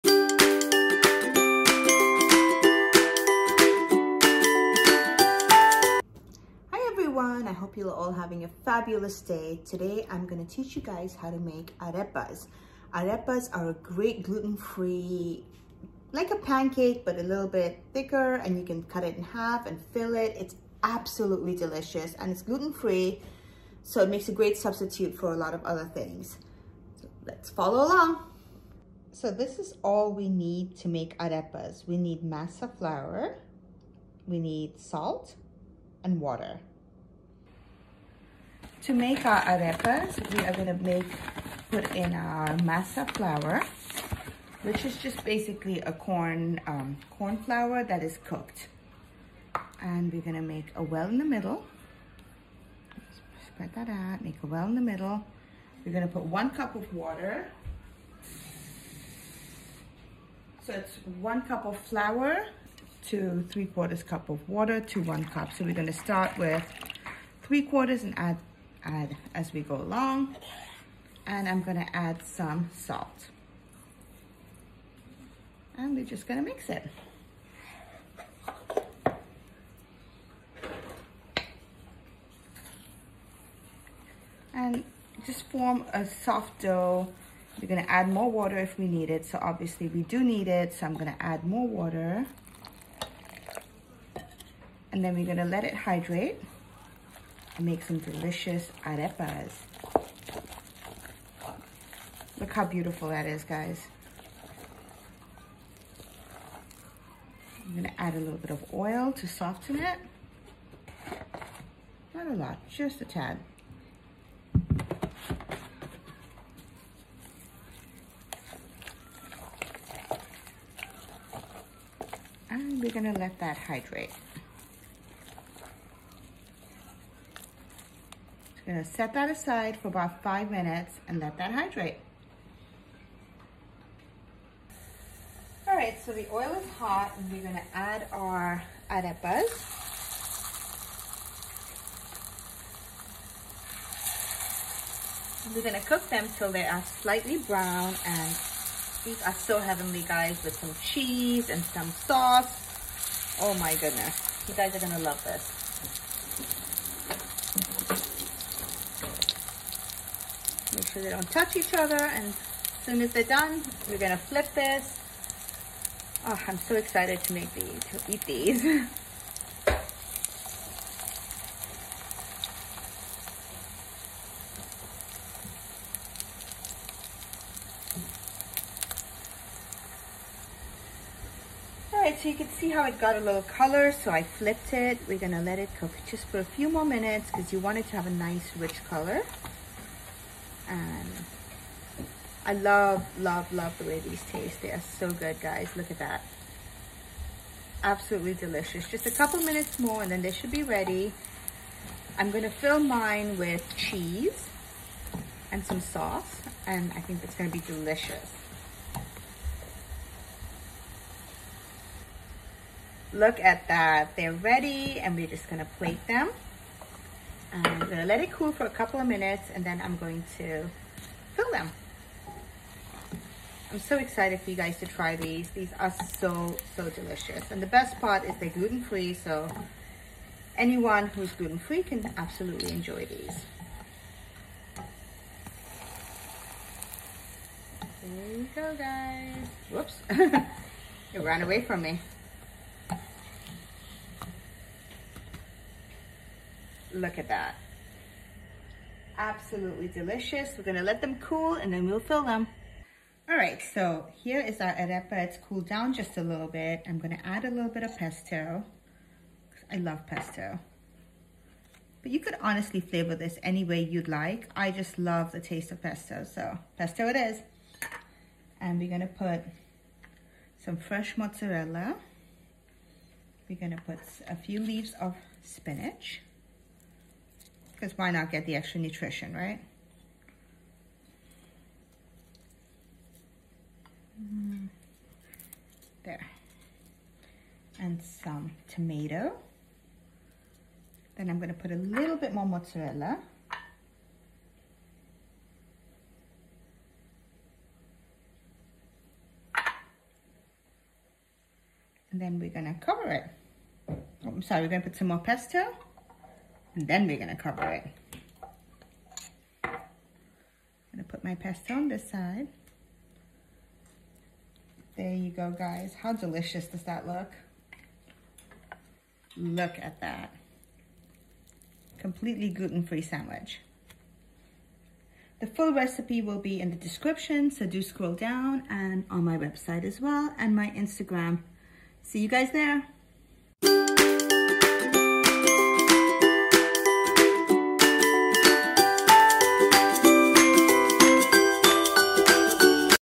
Arepas (corn pancakes)
Arepas are naturally gluten free. They are basically corn pancake sandwiches. They originated in Venezuela but they are also populate in Cuba, Spain & Latin America. They are so incredibly delicious and so versatile. You can basically fill them with anything your heart desires or anything you may have on hand.
Ingredients:

 2 cups arepa flour
(precooked cornmeal) called masarepa
2 teaspoons kosher salt
2 tablespoons vegetable oil
Fresh mozzarella
Tomatoes
Spinach
Pesto